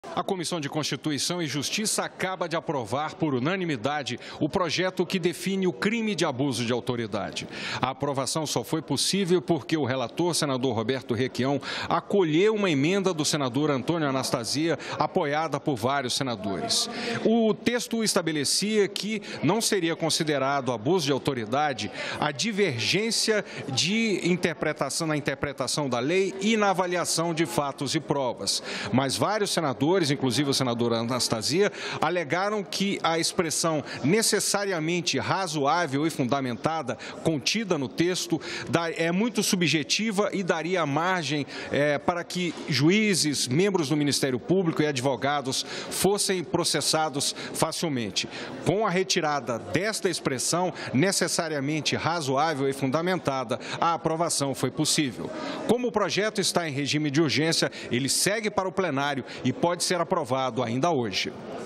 CCJ aprova por unanimidade projeto que altera lei de abuso de autoridade
A aprovação foi possível depois que o relator da matéria, senador Roberto Requião, acolheu uma emenda do senador Antônio Anastasia que retirou trecho do texto que trata sobre a possibilidade de puniçlão a juízes por divergências na interpretação da lei.